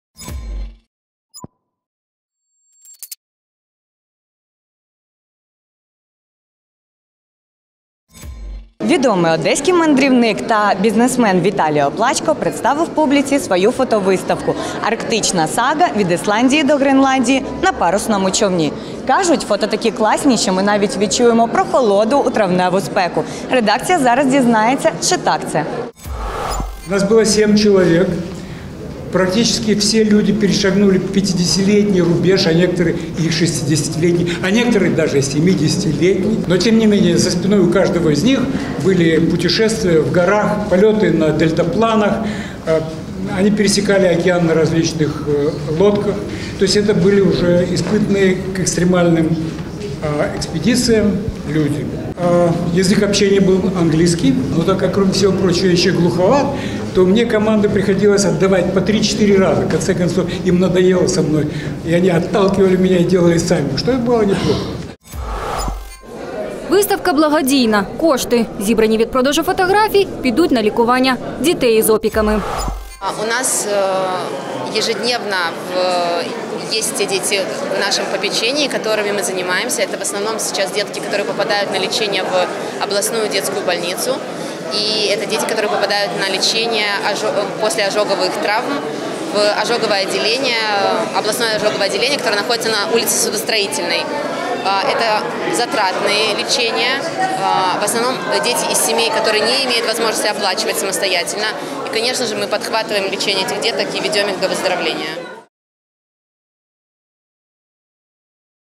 От Исландии в Гренландию на парусной лодке: в Одессе открылась благотворительная фотовыставка
Известный одесский меценат и путешественник представил в Музее западного и восточного искусства большую фотовыставку «Арктическая сага. От Исландии в Гренландию на парусной лодке».

Выставка — благотворительная. Собранные средства пойдут в фонд «Корпорация монстров», который помогает деткам.